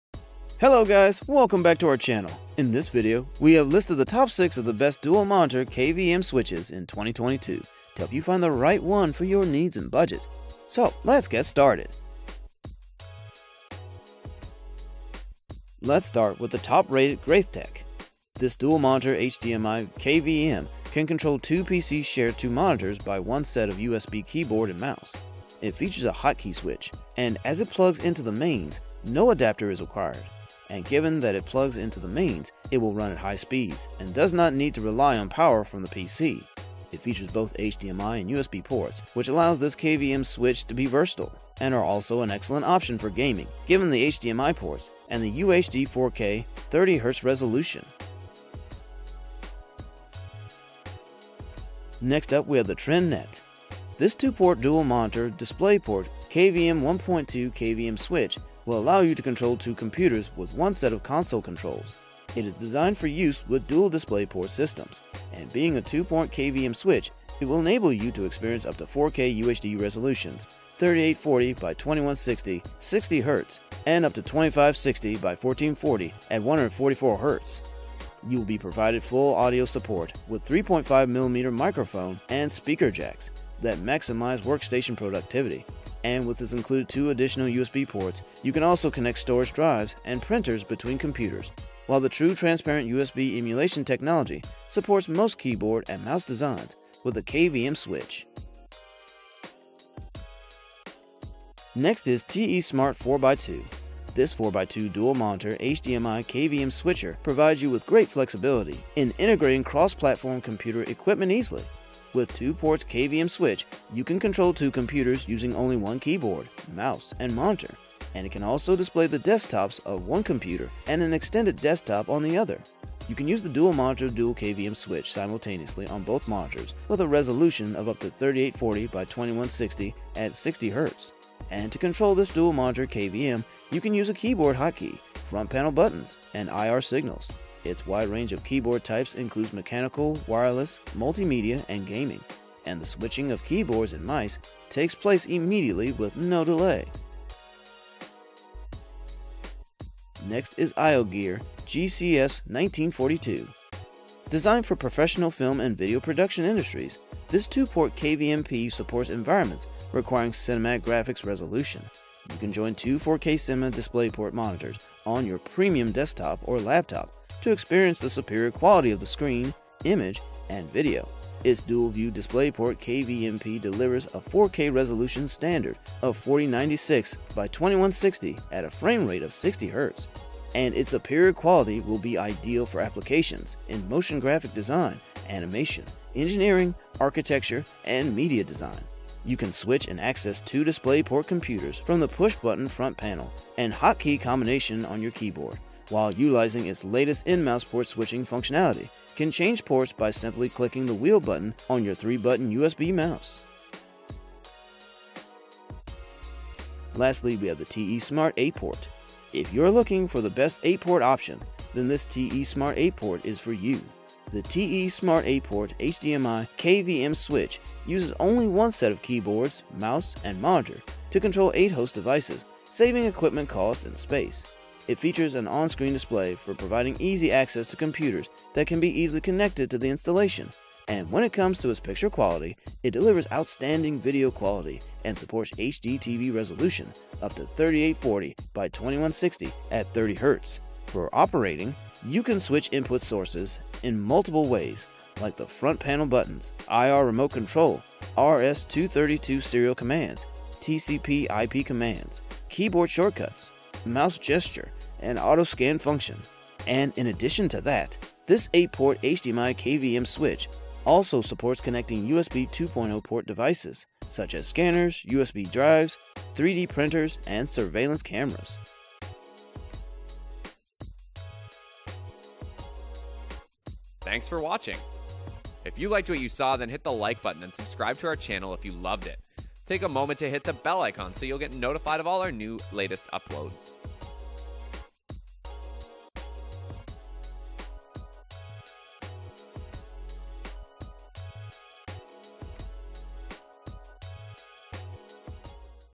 TOP 5: Best Dual Monitor KVM Switch 2022 | HDMI 2 Port Hub for Multiple Display Modes!
Check Out The Best Dual Monitor KVM Switch Here!
► Best Overall-GREATHTEK
► Best with Audio-TRENDnet
► Best Design-TESmart 4x2
► Best Hardware-IOGEAR GCS1942
► Best For 8 Port-TESmart 8 Port

VIDEO GUIDE
00:00 Best Dual Monitor KVM Switch
00:15 Best Overall
00:51 Best with Audio
01:41 Best Design
02:30 Best Hardware
03:26 Best For 8 Port

Let's start with the top-rated GREATHTEK. This Dual Monitor HDMI KVM can control 2 PC share two monitors by 1 set of USB keyboard & mouse. It features a hotkey switch, and as it plugs into the mains, no adapter is required. And given that it plugs into the mains, it will run at high speeds and does not need to rely on power from the PC. It features both HDMI and USB ports, which allows this KVM switch to be versatile. And are also an excellent option for gaming given the HDMI ports and the UHD 4K @ 30Hz resolution.

Next up, we have the TRENDnet. This 2-port Dual monitor DisplayPort KVM 1. 2 KVM switch will allow you to control two computers with one set of console controls. It is designed for use with Dual DisplayPort systems. And being a 2-port KVM switch, it will enable you to experience up to 4K UHD resolutions 3840 x 2160 @ 60Hz and up to 2560 x 1440 @144 Hz. You will be provided full audio support with 3. 5mm Microphone and speaker jacks that maximize workstation productivity. And with its included two additional USB ports, you can also connect storage drives and printers between computers. While its True Transparent USB emulation technology supports most keyboard and mouse designs with the KVM switch.

Next is TESmart 4x2. This 4x2 Dual Monitor HDMI KVM Switcher provides you with great flexibility in integrating cross-platform computer equipment easily. With the two ports KVM switch, you can control two computers using only one keyboard, mouse, and monitor. And it can also display the desktops of one computer and an extended desktop on the other. You can use the Dual Monitor Dual KVM Switch simultaneously on both monitors with a resolution of up to 3840x2160 @ 60Hz. And to control this Dual Monitor KVM, you can use a keyboard hotkey, front panel buttons, and IR signals. Its wide range of keyboard types includes mechanical, wireless, multimedia, and gaming. And the switching of keyboards and mice takes place immediately with no delay.

Next is IOGEAR GCS1942. Designed for professional film and video production industries, this 2-Port KVMP supports environments requiring cinematic graphics resolution. You can join two 4K Cinema DisplayPort monitors on your premium desktop or laptop to experience the superior quality of the screen, image, and video. Its Dual View DisplayPort KVMP delivers a 4K resolution standard of 4096 x 2160 at a frame rate of 60Hz. And its superior quality will be ideal for applications in motion graphic design, animation, engineering, architecture, and media design. You can switch and access two DisplayPort computers from the push-button front panel or hotkey combination on your keyboard. While utilizing its latest in mouse port-switching functionality can change ports by simply clicking the wheel button on your 3-button USB mouse.

Lastly, we have the TESmart 8 Port. If you are looking for the best 8 port option, then this TESmart 8 Port is for you. This TESmart 8 port HDMI KVM Switch uses only 1 set of keyboards, mouse, and monitor to control eight host devices, saving equipment cost and space. It features an on-screen display for providing easy access to computers that can be easily connected to the installation. And when it comes to its picture quality, it delivers outstanding video quality and supports HDTV resolutions up to 3840 * 2160 @ 30 Hz. For operating, you can switch input sources in multiple ways like via the front panel buttons, IR remote control, RS232 serial commands,